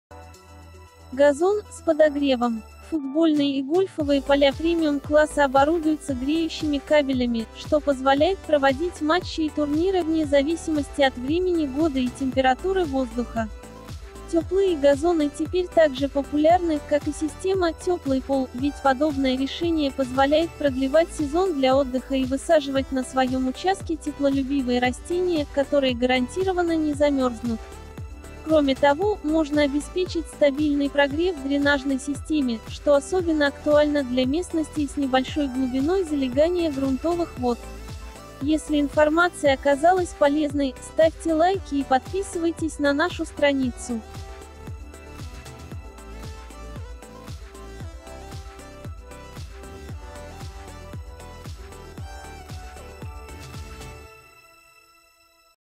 Газоны для футбольных и гольф полей с подогревом
Газоны с подогревом или теплые газоны. Вдохновляйтесь идеями и обращайтесь в нашу мастерскую ландшафтного дизайна.